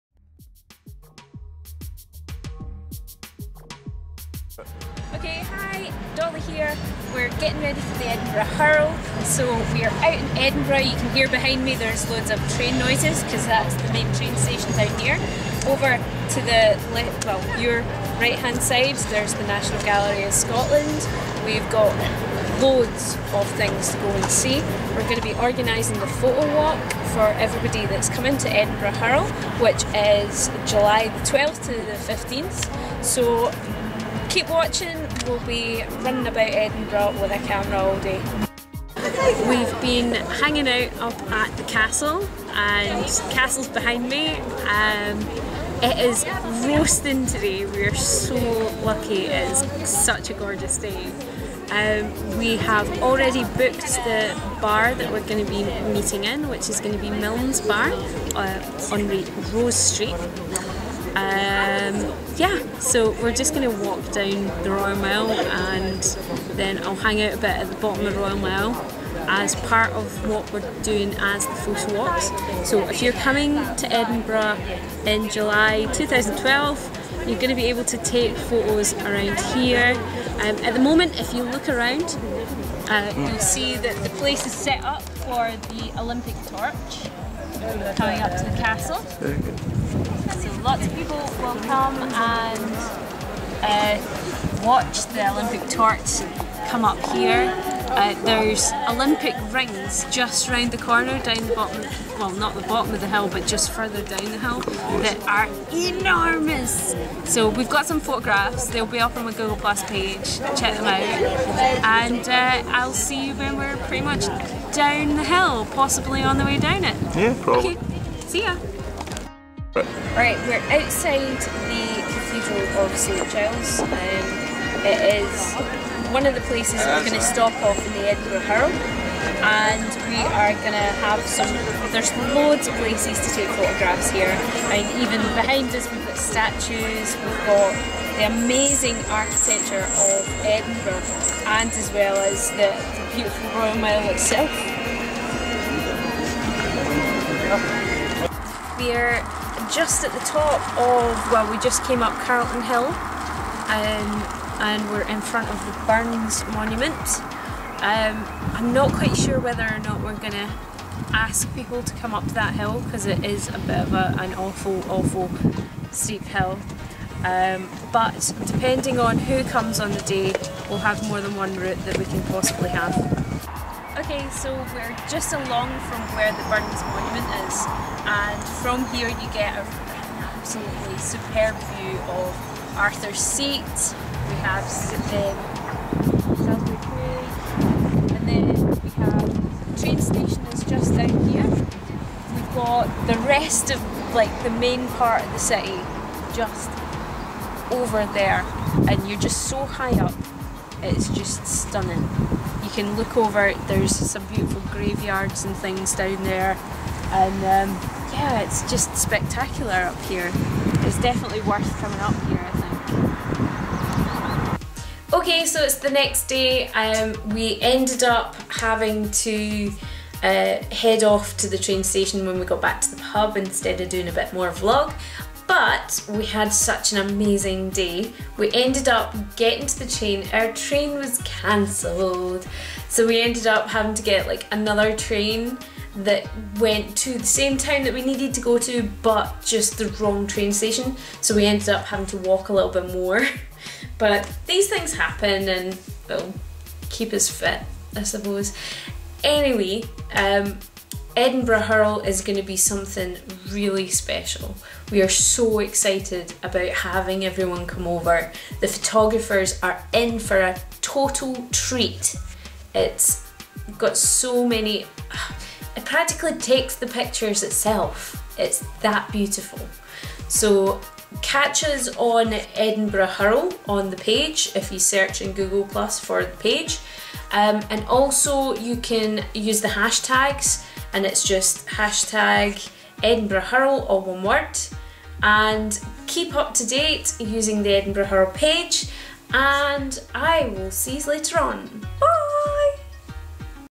Vlog - Getting ready for Edinburgh HIRL Photowalk
This was a walkthrough of the Edinburgh HIRL photowalk.

People from Google+ are meeting up in Scotland's capital city Edinburgh. During the meetup, we'll be taking a photowalk - an organised walk where the host of the country takes a group around some of the sights of the city.

Thanks to Steven Mcvicar for being my cameraman...

Can't wait to see you at the HIRL!